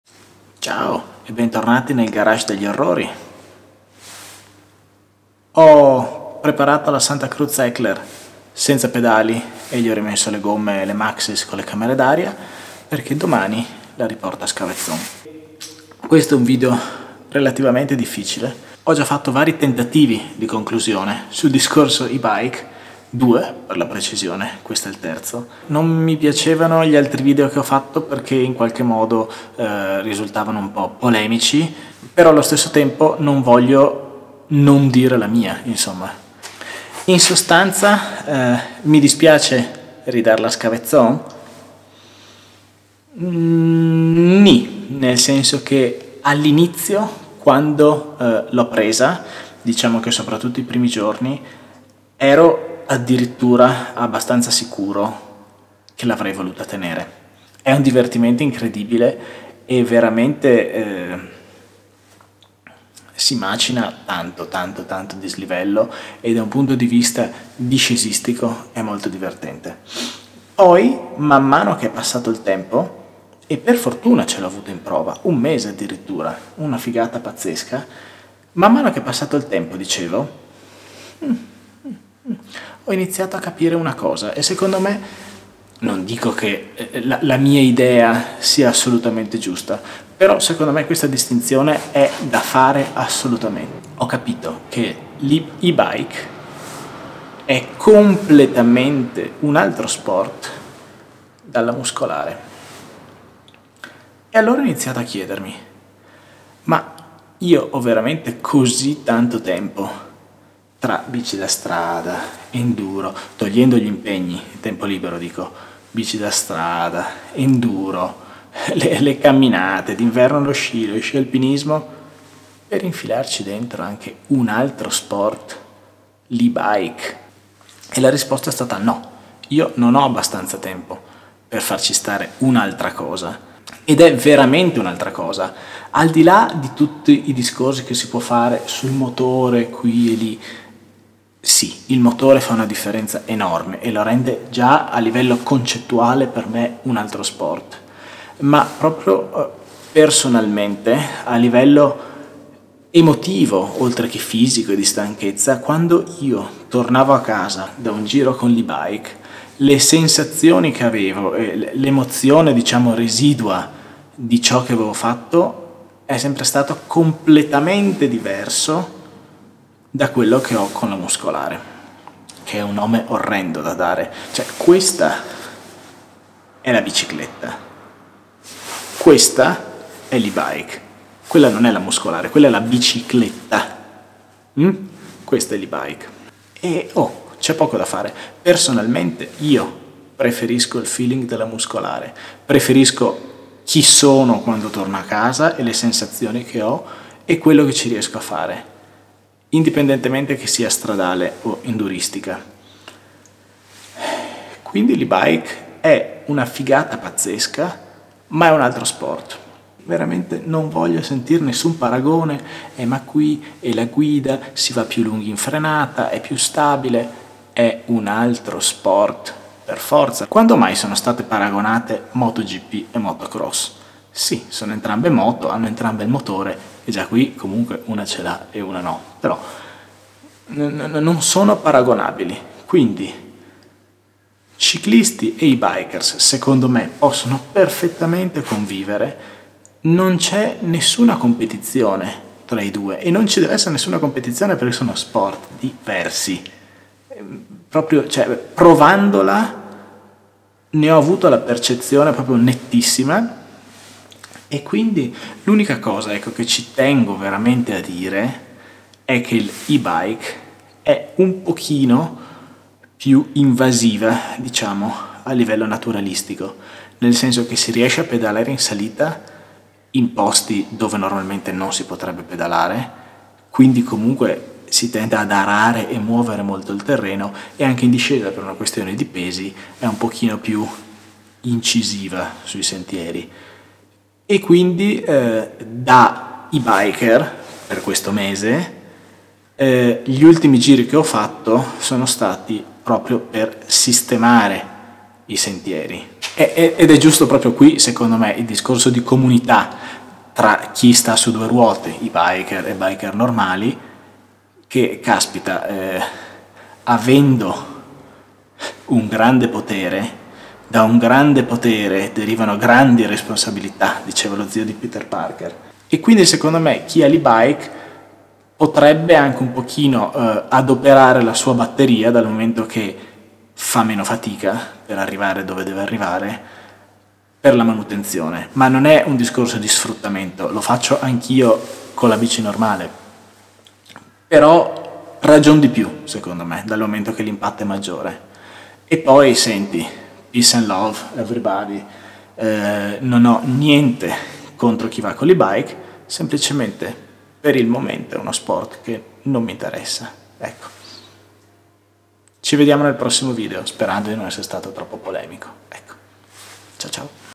⚡️ HECKLER BYE BYE ⚡️- considerazioni dopo un mese di e-bike
Il mese di prova con la Santa Cruz Heckler CC X01 RSV (che nome lungo!) è scaduto, l'ho riportata da Scavezzon ed ecco qualche considerazione finale dopo il suo utilizzo